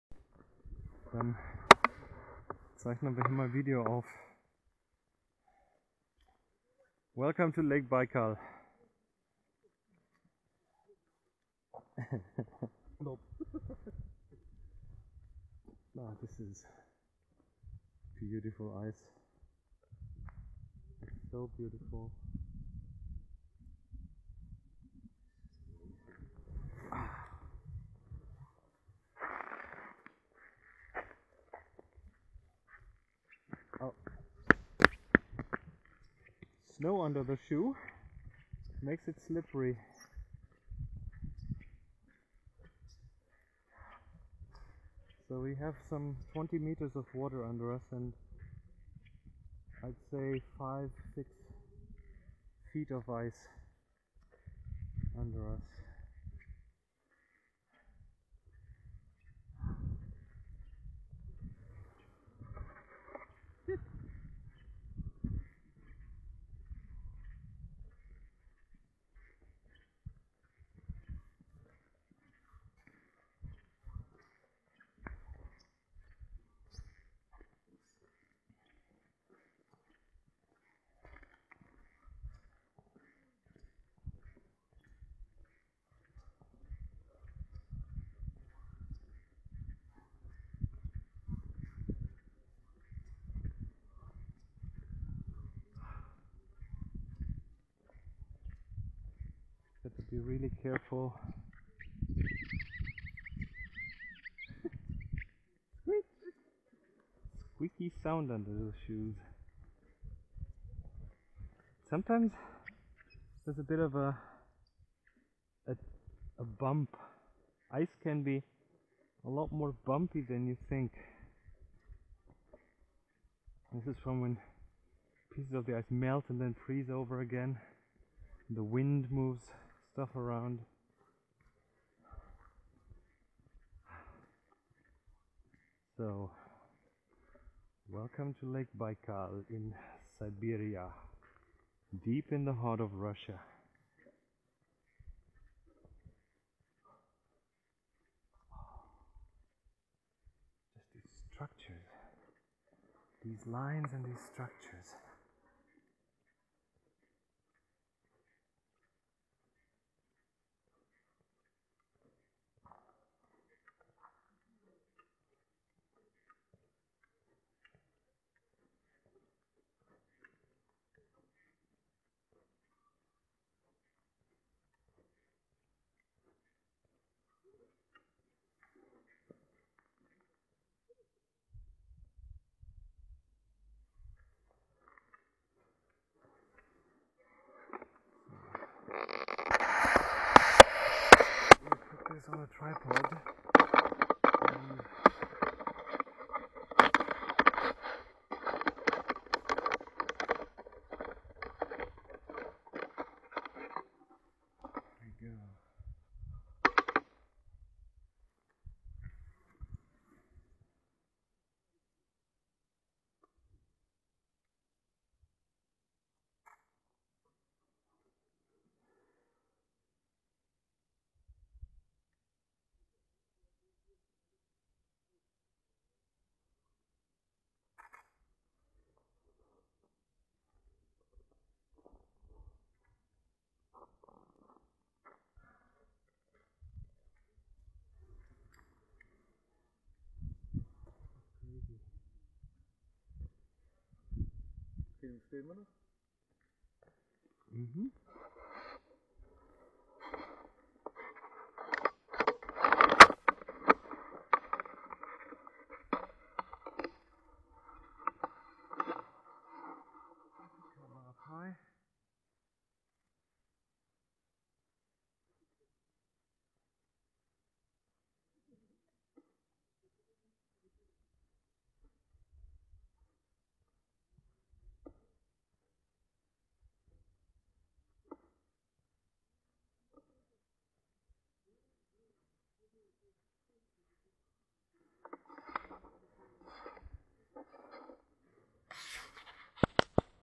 360 Walk Lake Baikal - A Photo Tour with Chris Marquardt
Black ice on Lake Baikal. We walk and take some photos.